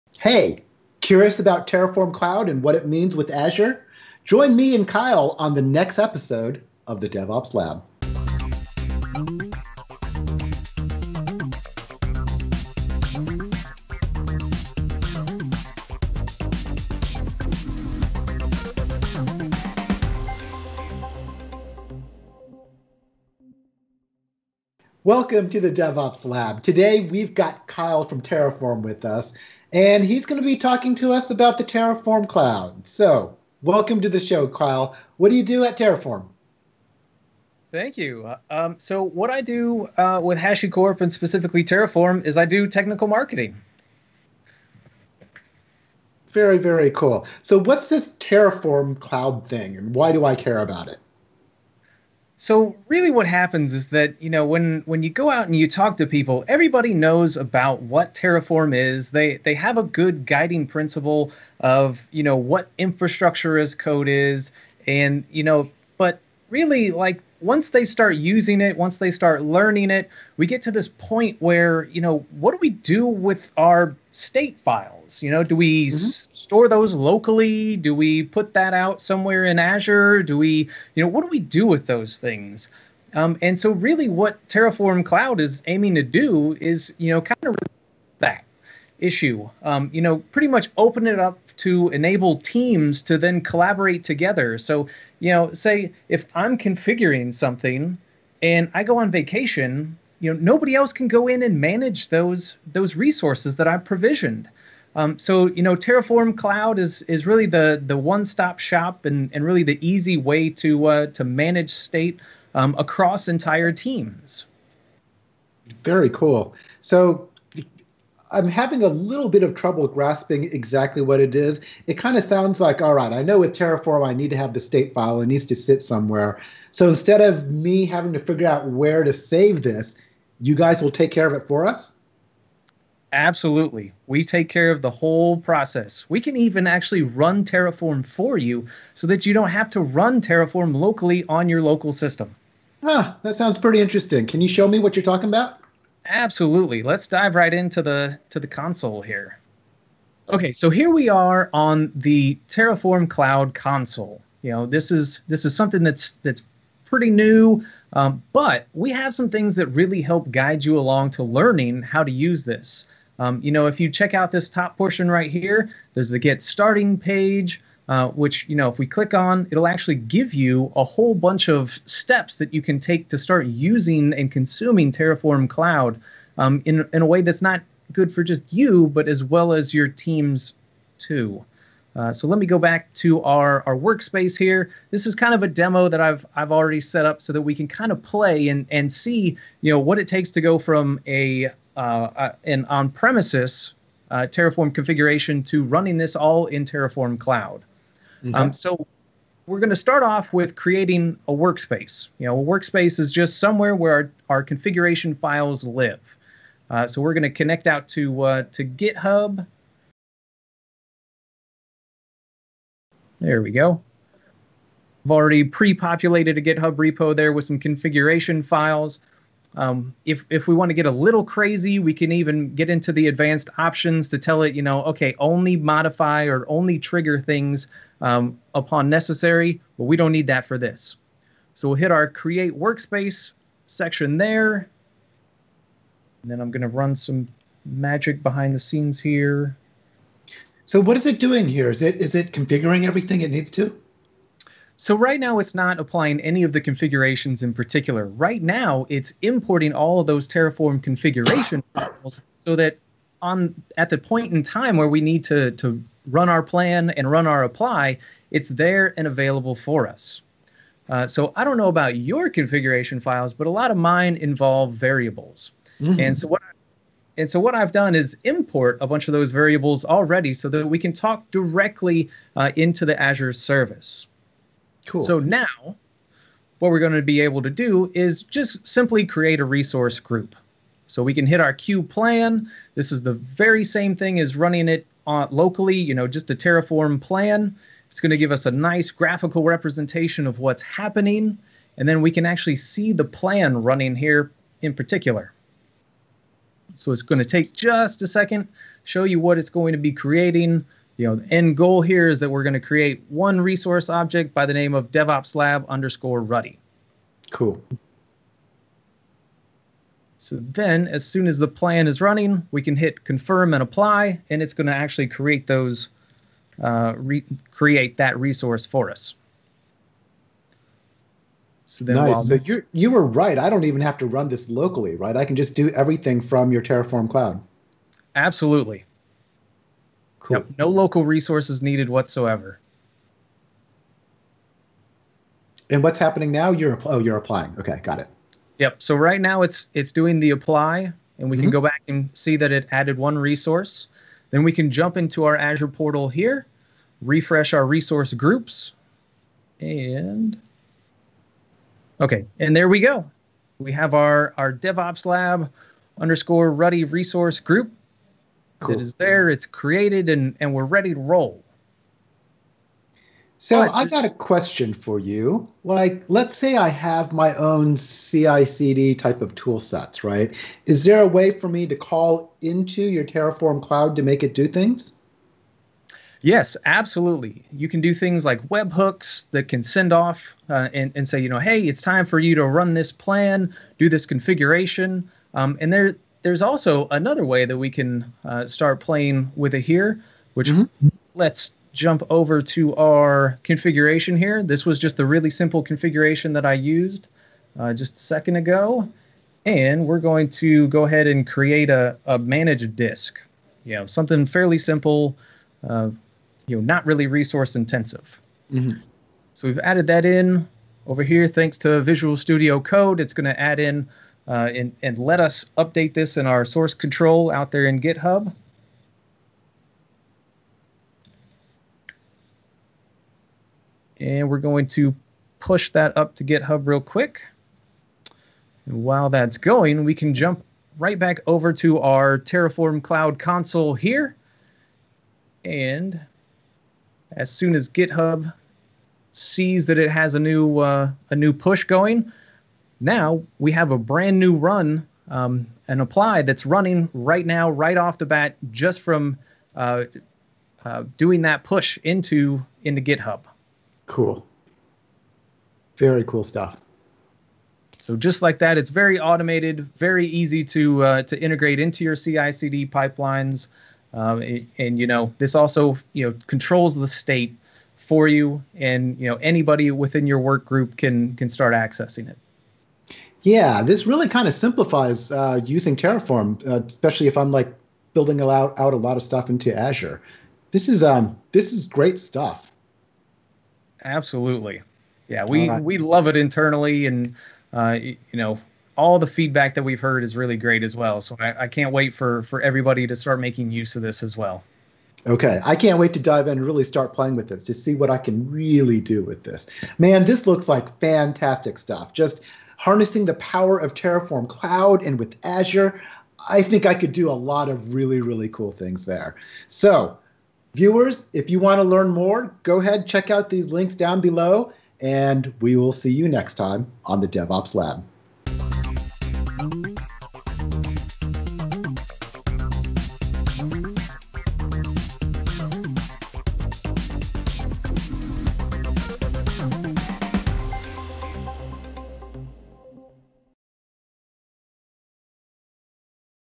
Remote State Management with Terraform Cloud
HashiCorp's Terraform Cloud is a SaaS platform where we can more easily collaborate on Terraform configurations through remotely stored, versioned, and shared Terraform state files. Check out how easy it is to get started deploying your Azure resources!

Jump To:
[03:05] - Demo: Creating a Terraform Cloud workspace to create and manage an Azure Resource Group
[06:32] - Demo: Updating the Terraform configuration to automatically trigger the creation of an Azure Managed Disk